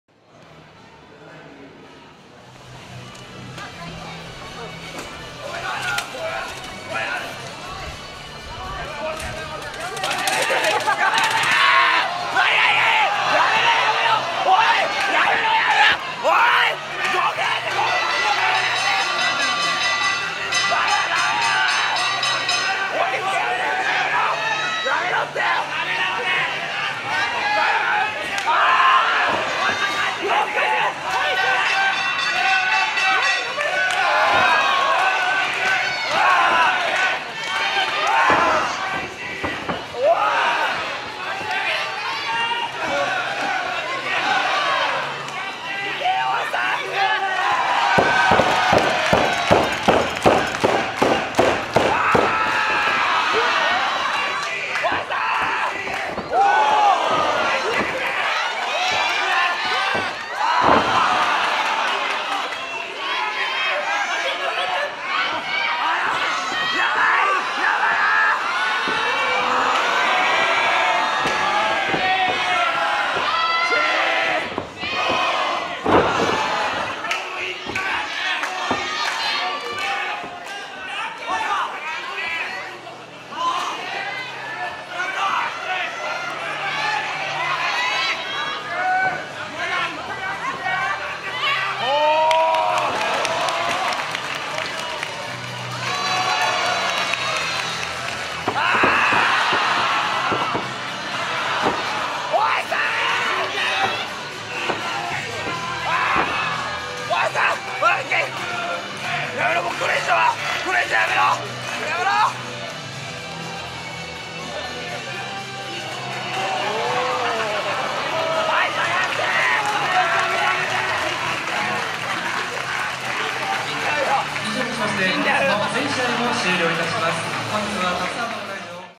謎の黒覆面がフランク篤を強襲!!
2016年12月30日後楽園ホール大会終了後、謎の黒覆面がフランク篤を強襲!!
黒覆面の正体や如何に!!

ニコニコ動画大日本プロレス公式ch
橋本大地・なんだ神田プロダクション